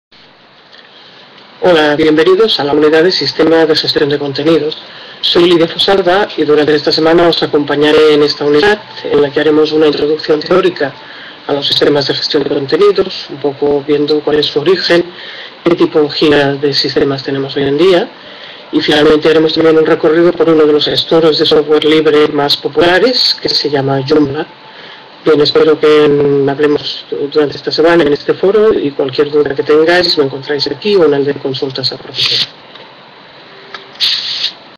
MDDOL Presentación Unidad 7.3. Sistemas de Gestión de Contenidos
Presentación de la Unidad 7.3. Sistemas de Gestión de Contenidos con Lídia Fosalba
Máster Universitario en Documentación Digital
Universidad Pompeu Fabra